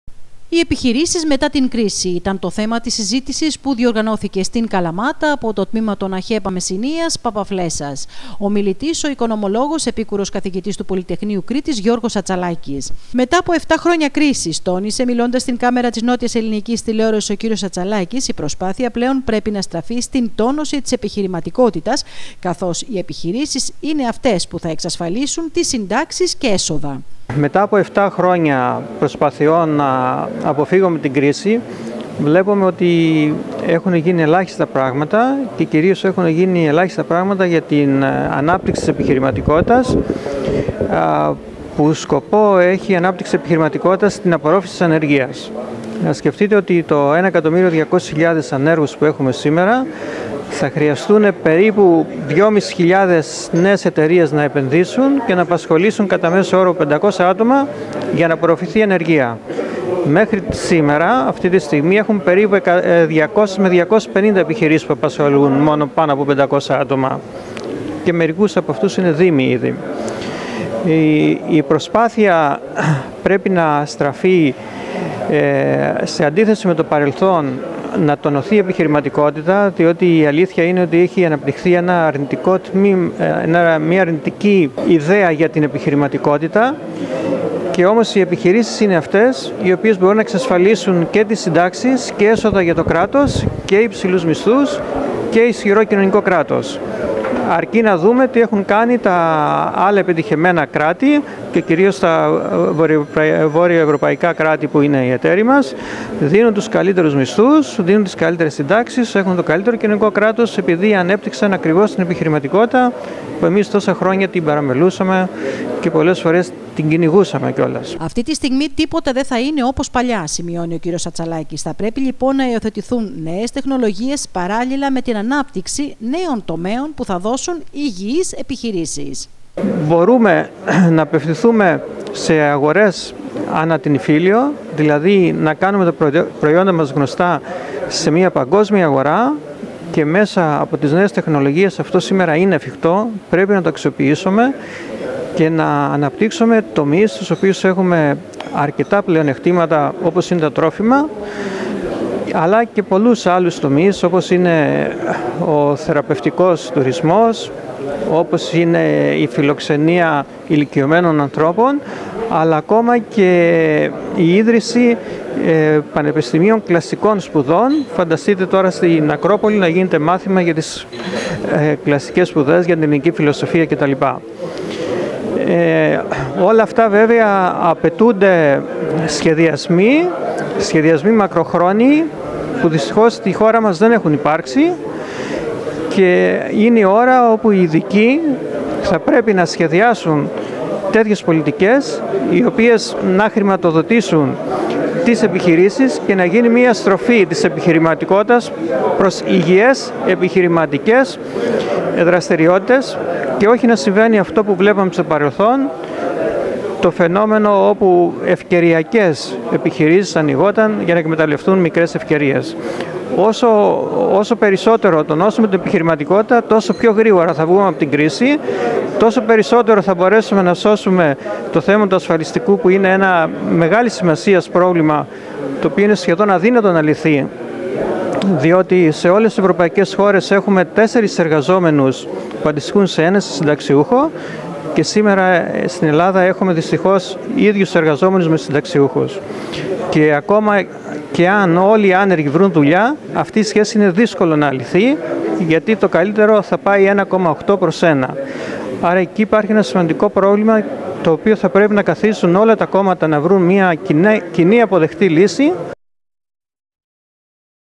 AXEPANS MESSHNIAS 27 1
Οι επιχειρήσεις μετά την κρίση ήταν το θέμα της εκδ'ηλωσης που διοργάνωσε στην Καλαμάτα το τμήμα των Αχέπανς Μεσσηνίας και η περιφερειακή ενότητα Μεσσηνίας . Ομιλητής ο  Επίκουρος Καθηγητής του Πολυτεχνείου Κρήτης Γιώργος Aτσαλάκης.